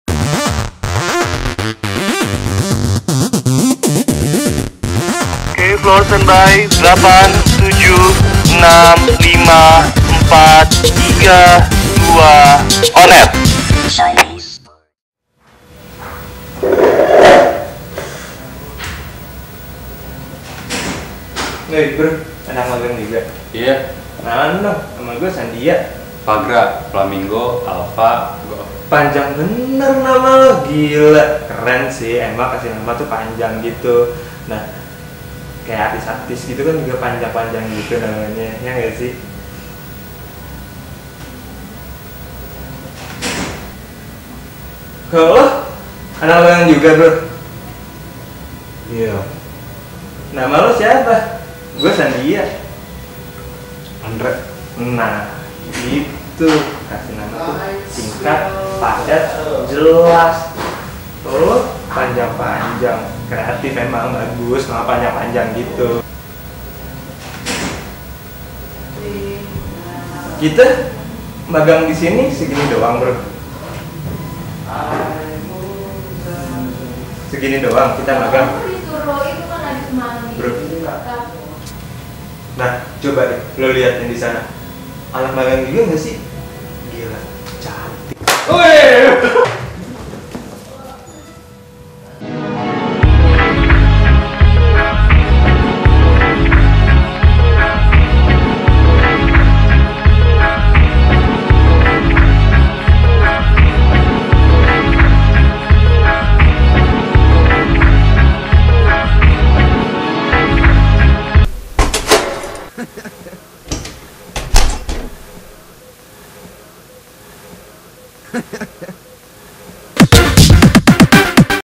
ON AIR Eps. 1 - Baru Kenalan
Percakapan yang terjadi antara anak-anak magang baru di BINUS TV. Eh, ada satu hal yang ngeganggu percakapan mereka. Apaan tuh?

Casts:
Anak Magang